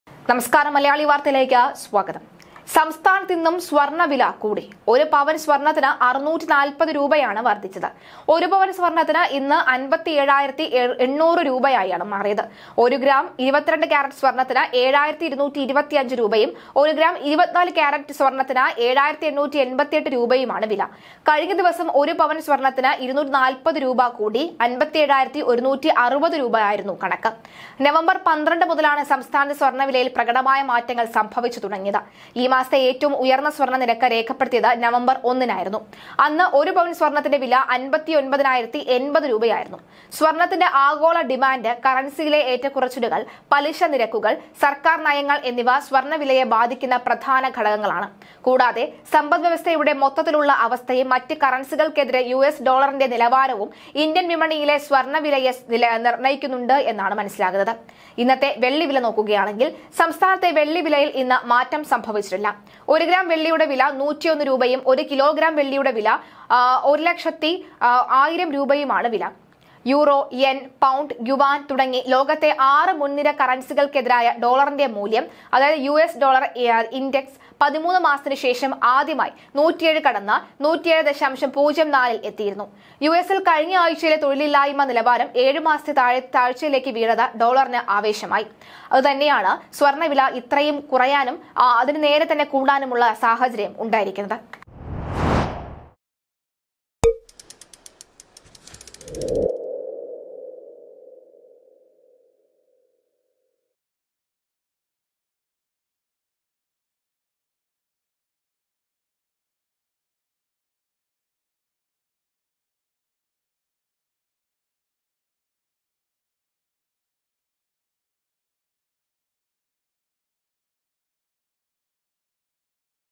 GOLD ദൈവമേ കൈവിട്ടോ
​ സ്വർണവിലയിൽ
വമ്പൻ ട്വിസ്റ്റ്..!
ഒറ്റ ദിവസംകൊണ്ട് വമ്പൻ മാറ്റം
വീണ്ടും നിരാശ,സംഭവിക്കുന്നത്